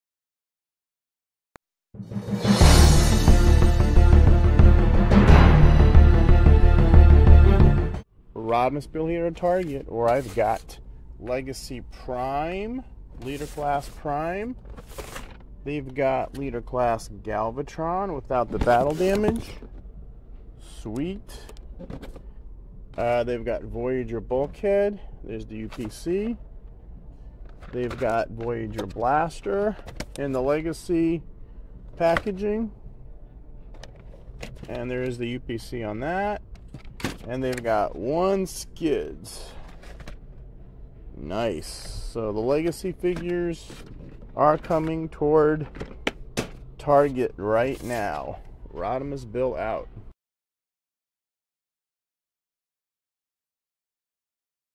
Transformers Legacy figures hitting stores now at Target! *Rodimusbill New Toy Sighting"
Figures listed below...
Legacy.
Leader class Laser Optimus prime
Leader class Galvatron
Voyager Prime Universe Bulkhead
Voyager Blaster and Eject
Deluxe Autobot Skids

Studio Series
Leader Class Coronation Starscream